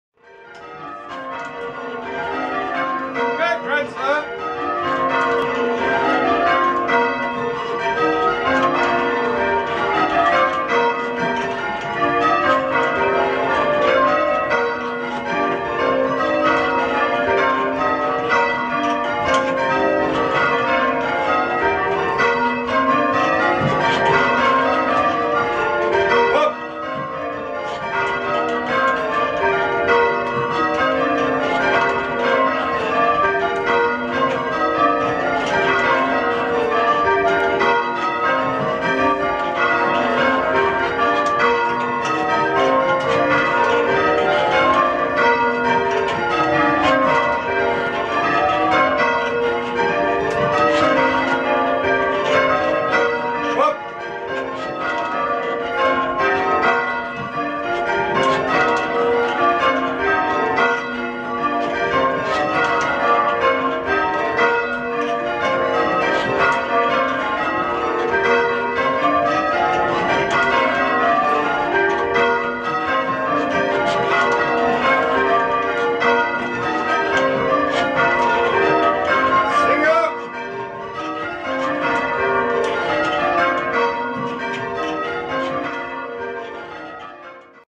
Oxford Bellringing Outing - St Ebbe
Footage of the PUBS ringers and friends on their 2014 summer outing.